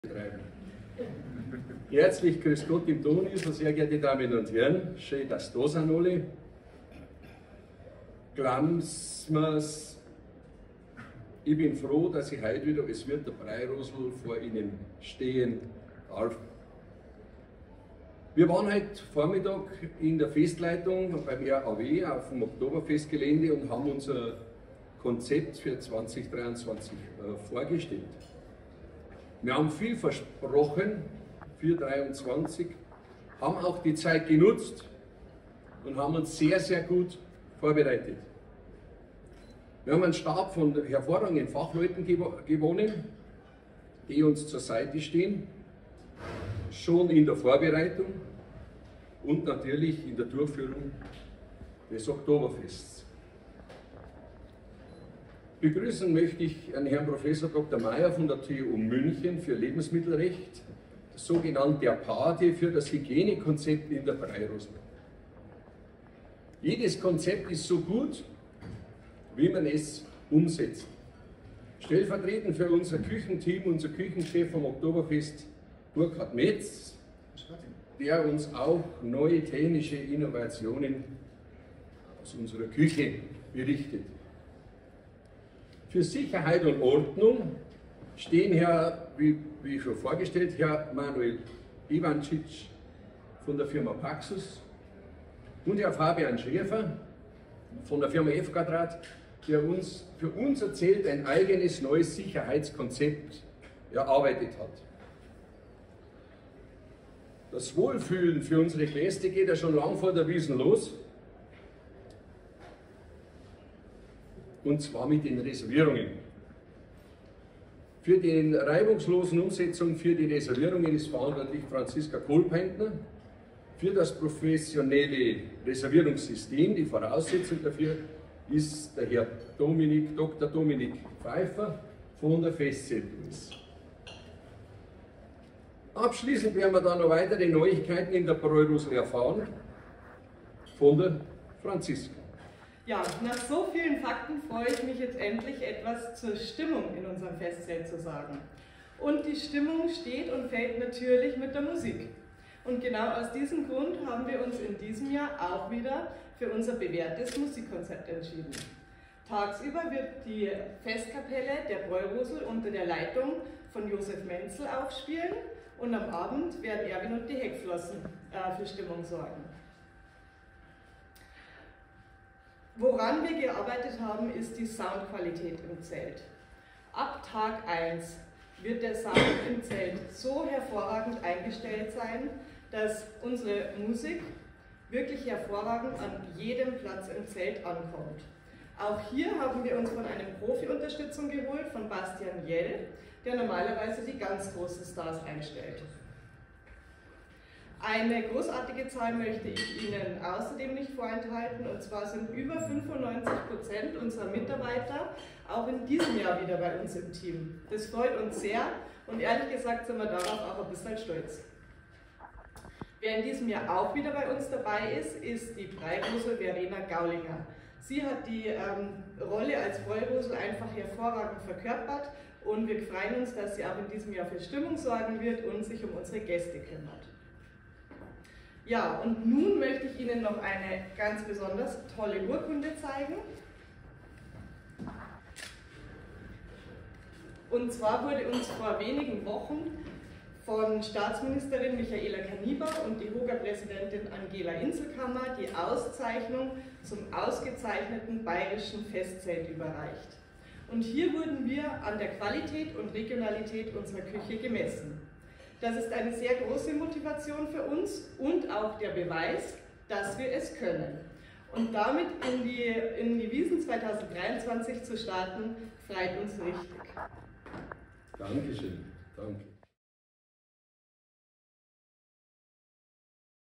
neues von der Bräurosl auf dem Oktoberfest 2023 - vorgestellt im Donisl von Wirt Peter Reichert
Reichert: "Glauben sie mir, Ich bin froh, dass Sie heut wieder als Wirt der Bräurosl wieder vor ihnen stehen darf. Wir waren heut Vormittag in der Festleitung und beim RAW auf dem Oktoberfest Gelände und haben unser Konzept für 2023 vorgestellt. Wir haben viel versprochen für 2023 haben auch die Zeit genutzt und haben uns sehr sehr gut vorbereitet. Wir haben einen Stab von hervorragenden Fachleuten gewonnen, die uns zur Seite stehen, schon in der Vorbereitung und natürlich in der Durchführung des Oktoberfests."

Für die Reservierung ist verantwortlich Franziska Kohlpaintner  " ...freue ich mich, jetzt endlich etwas zur Stimmung in unserem Festzelt zu sagen. Und die Stimmung steht und fällt natürlich mit der Musik. Und genau aus diesem Grund haben wir uns in diesem Jahr auch wieder für unser bewährtes Musikkonzept entschieden. Tagsüber wird die Festkapelle der Bräurosl unter der Leitung von Josef Menzl aufspielen und am Abend werden "Erwin und die Heckflossen" für Stimmung sorgen. Woran wir gearbeitet haben, ist die Soundqualität im Zelt. Ab Tag eins wird der Sound im Zelt so hervorragend eingestellt sein, dass unsere Musik wirklich hervorragend an jedem Platz im Zelt ankommt."